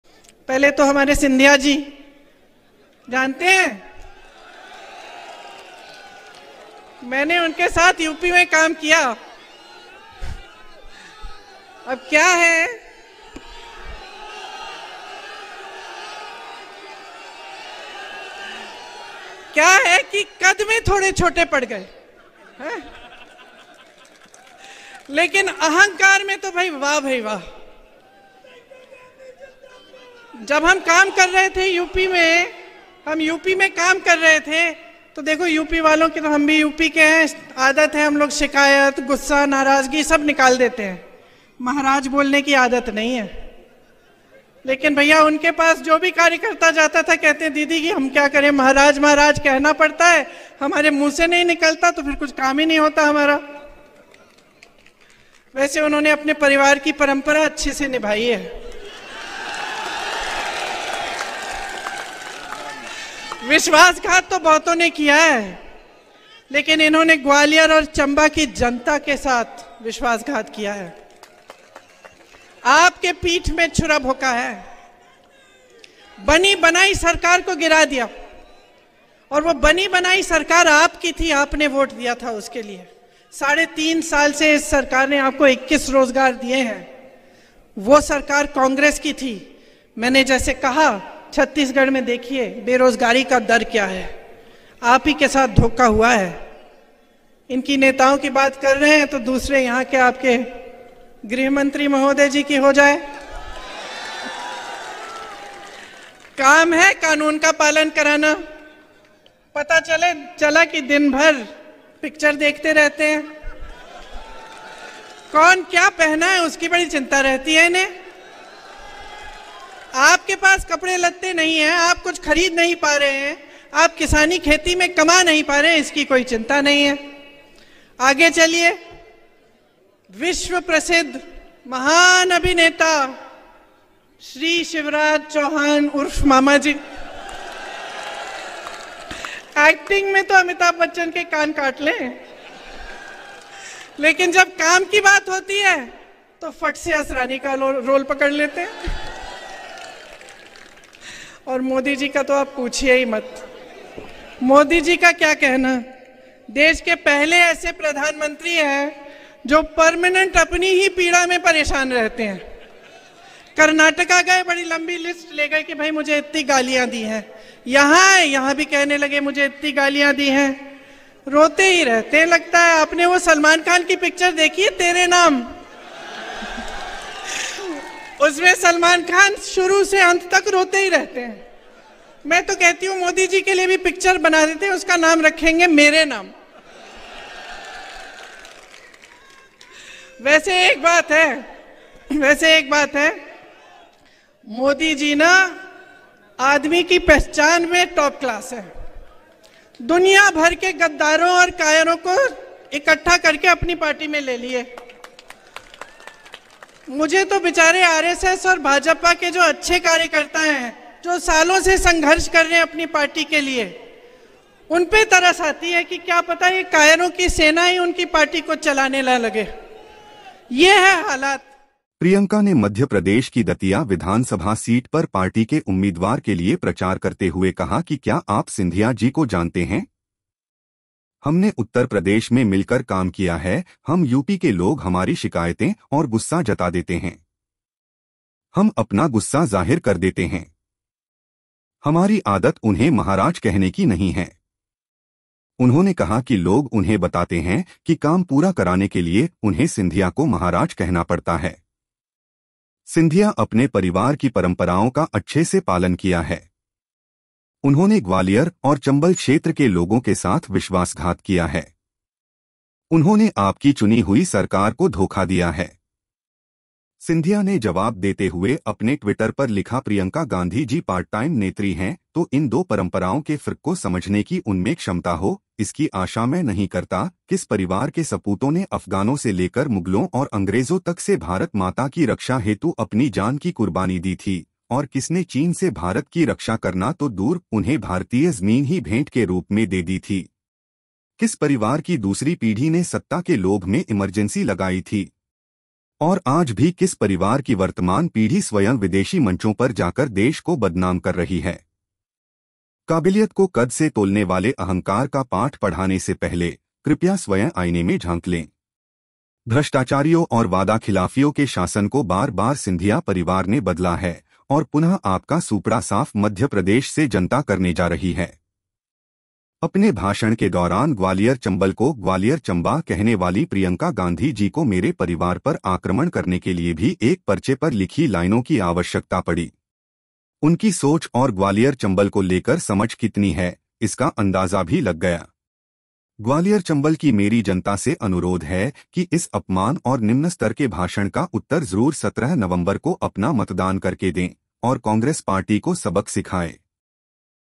सिंधिया महाराज पर प्रियंका गाँधी का बड़ा हमला, दतिया की चुनावी जनसभा से सुनिए क्या बोलीं?
सिंधिया महाराज पर प्रियंका गाँधी का बड़ा हमला, दतिया की चुनावी जनसभा से सुनिए क्या बोलीं? इसके बाद ज्योतिरादित्य सिंधिया ने भी पलटवार करते हुए प्रियंका पर जमकर निशाना साधा, देखिये पूरा विडियो

Priyanka was speaking at a political gathering to support Rajendra Bharti, a Congress candidate in the Datia Assembly seat of Madhya Pradesh. The elections for this seat are scheduled for November 17.

During her speech, Priyanka criticized Jyotiraditya Scindia, who switched to the BJP. She mentioned her past collaboration with Scindia in Uttar Pradesh, highlighting his arrogance. Priyanka claimed that people in Uttar Pradesh often felt compelled to address Scindia as 'maharaj' (a term of respect or royalty). According to her, party workers complained that their tasks were only fulfilled when they addressed him this way. Despite this, Priyanka acknowledged that Scindia faithfully followed his family's traditions. She accused him of overthrowing the government that the people had voted for.